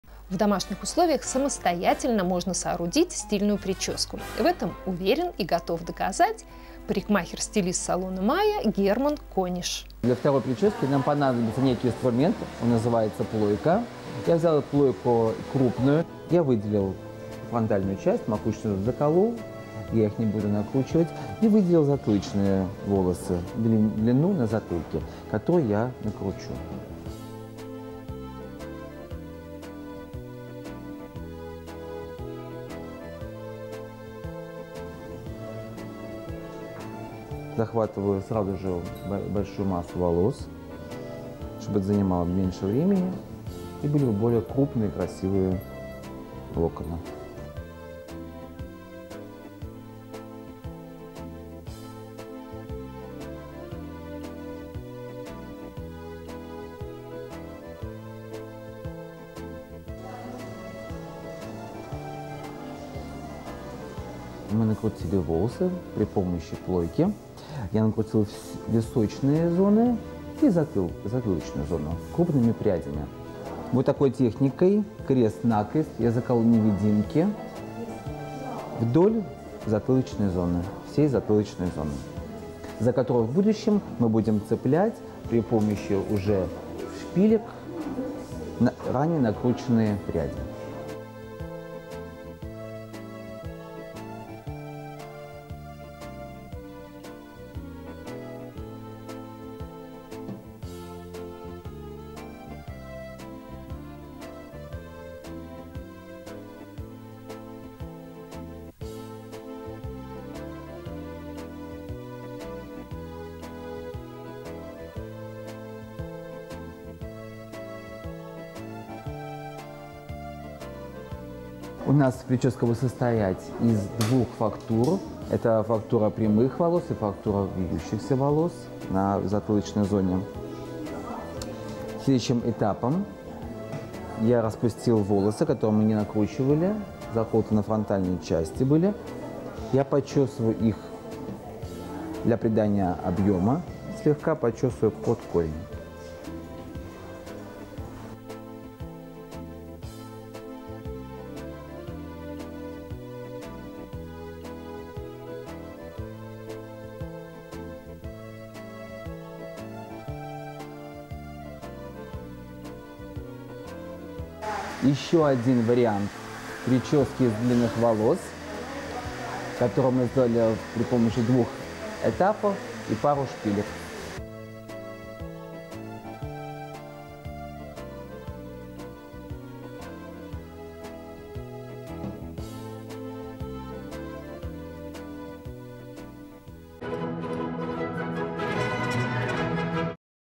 Прическа в домашних условиях от Германа Кониша
В домашних условиях самостоятельно можно соорудить стильную прическу. Это утверждает и готов доказать парихмахер-стилист салона Майя Герман Кониш.